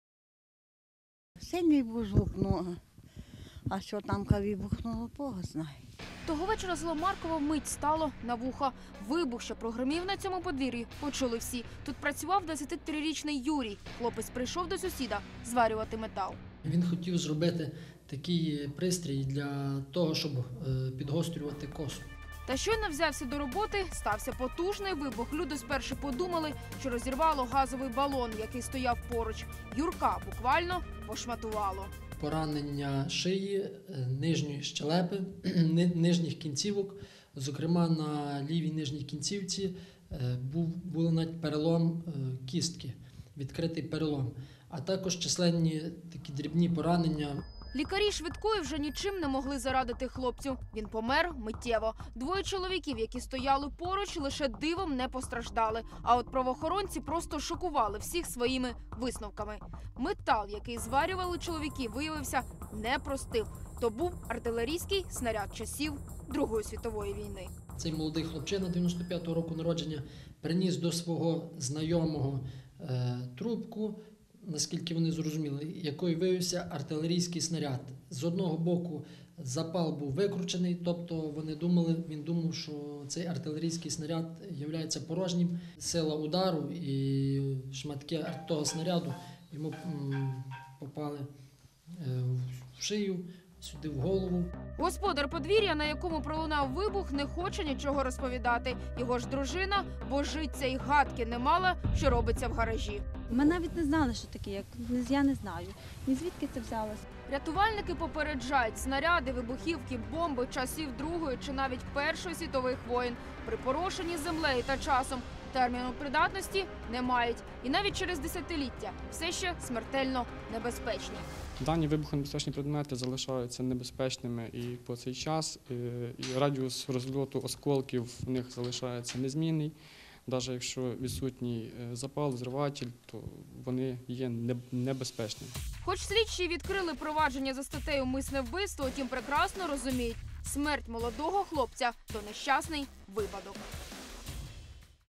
На Івано-Франківщині стався вибух
Того вечора село стало на вуха. Вибух, що прогримів на подвір'ї почули всі. Хлопець прийшов до сусіда-зварювати метал . Та щойно взявся до роботи-стався вибух. Лікарі нічим вже не могли зарадити. А от правоохоронці шокували висновками: метал, який зварювали не був простим. Це був артилерійський снаряд. Господар подвір'я, на якому пролунав вибух, не хоче спілкуватися. Рятувальники попереджають, що бомби, снаряди та вибухівки попередніх війни не мають терміну придатності і через 10-ліття небезпечні.